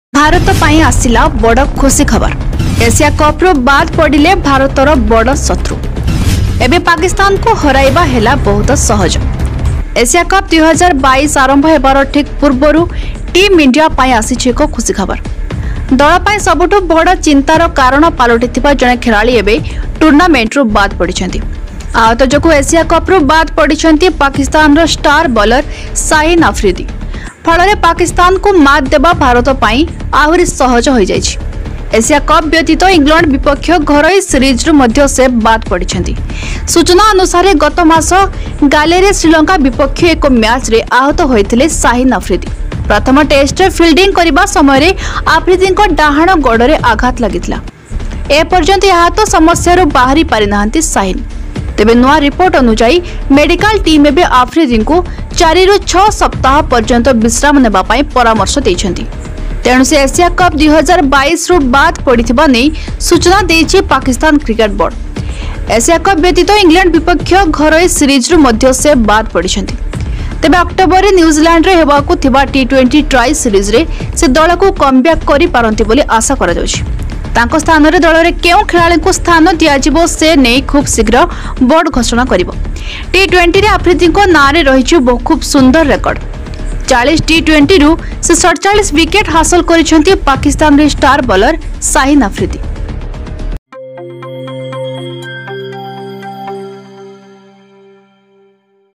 IND vs ZIM 2nd ODI highlights| Shaheen afridi ruled out of asia cup 2022 due to injury|cricket news
Shaheen afridi ruled out of asia cup 2022 due to injury | ଏସିଆ କପରୁ ଶାହୀନ ଆଫ୍ରିଦି ହେଲେ ଆଉଟ.